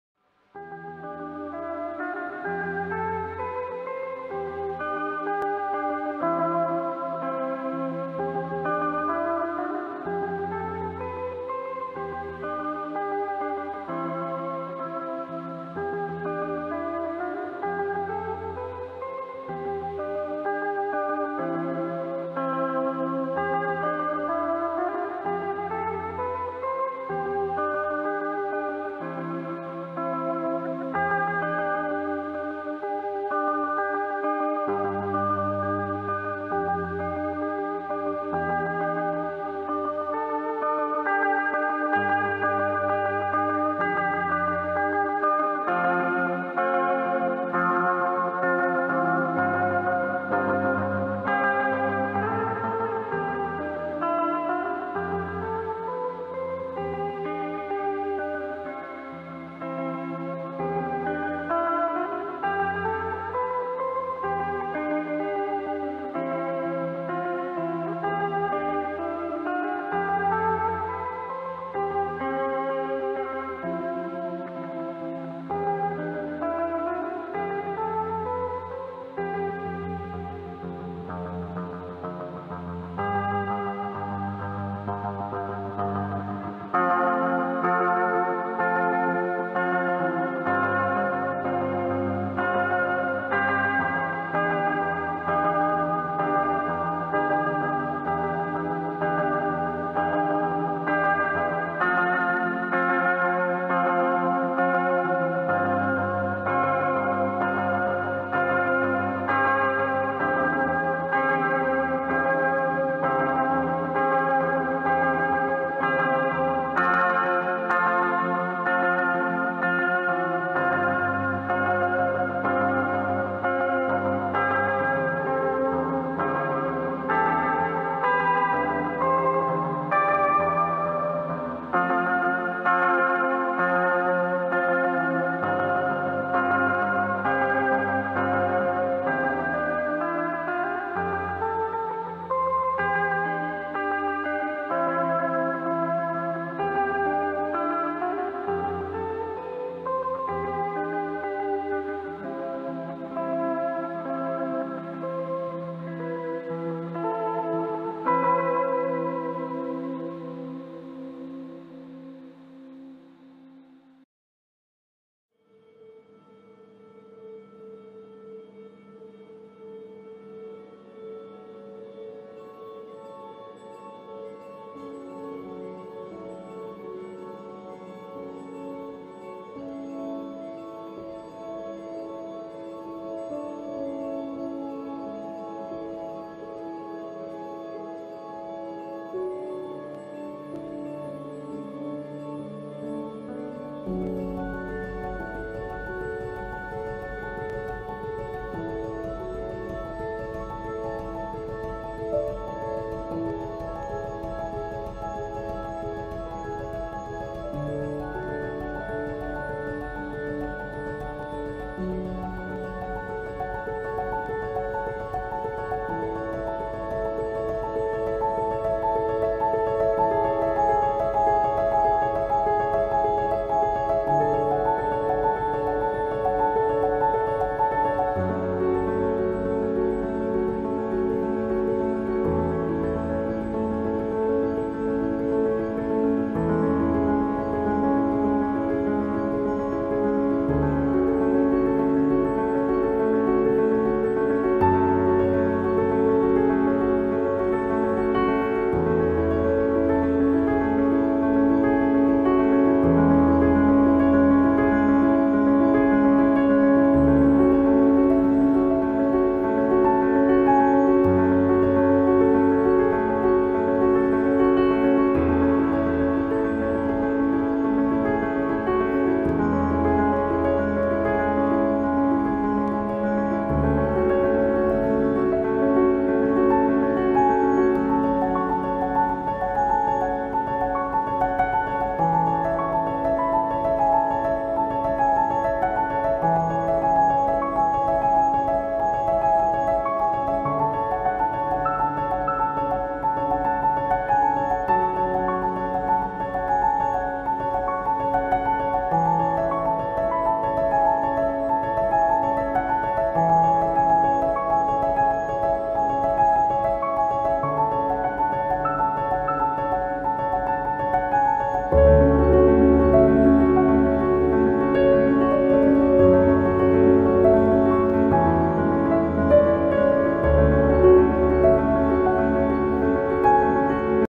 The life of charlotte lotte reigner: Animation
silhouette animation of charlotte lotte reigner life